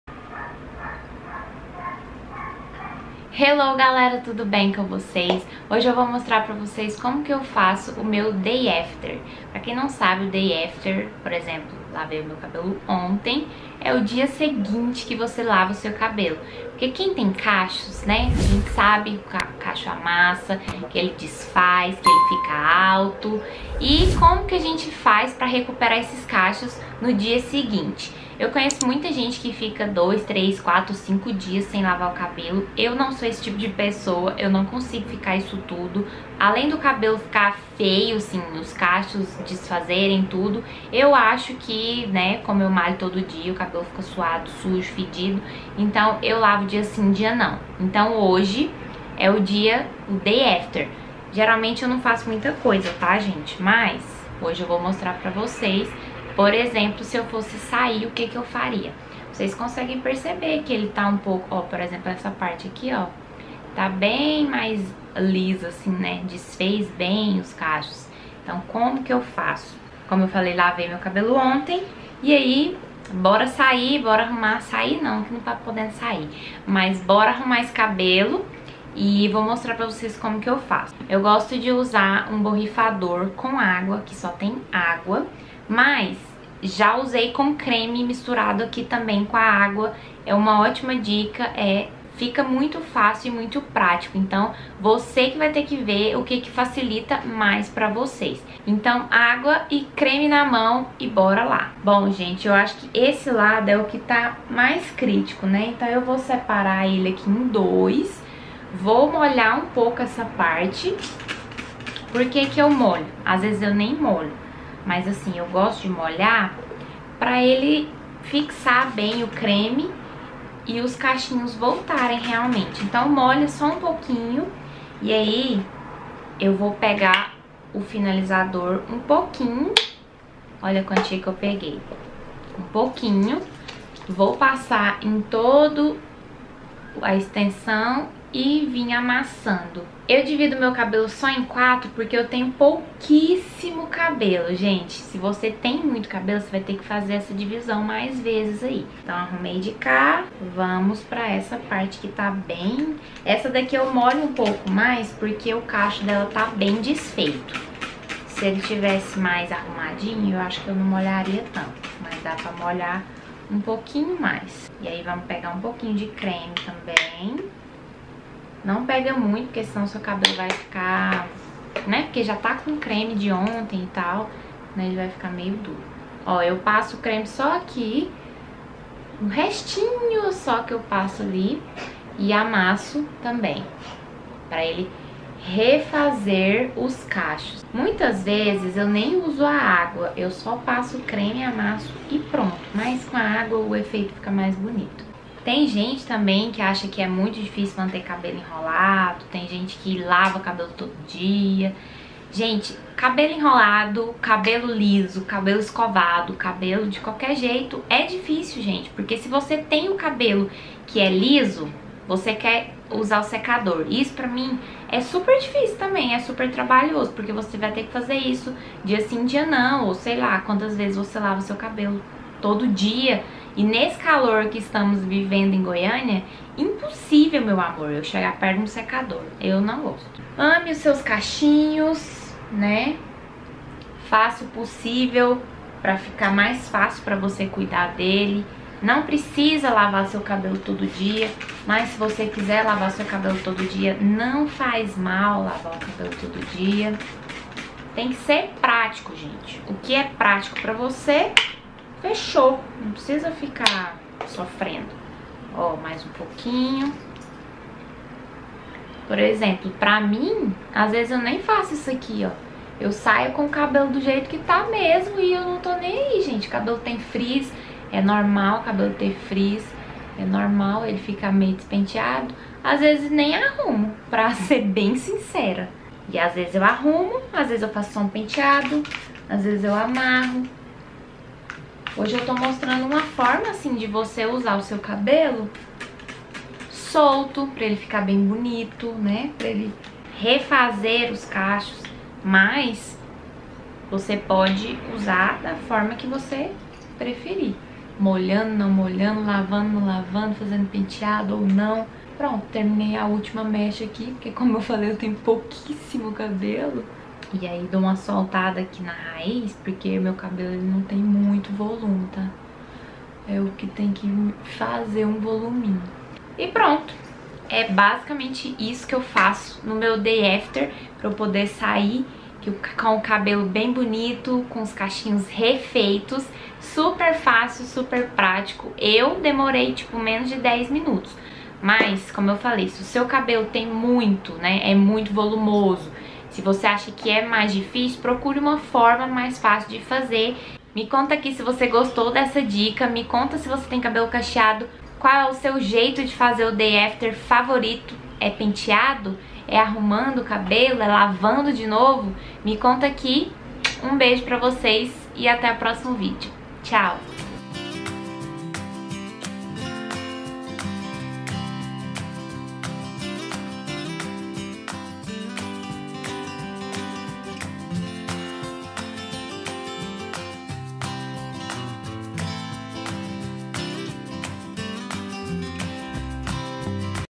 COMO FAÇO MEU DAY AFTER
Mostrei nesse vídeo o que eu faço um dia após ter lavado o cabelo. Espero que gostem, me sigam no insta que estou por lá todos os dias.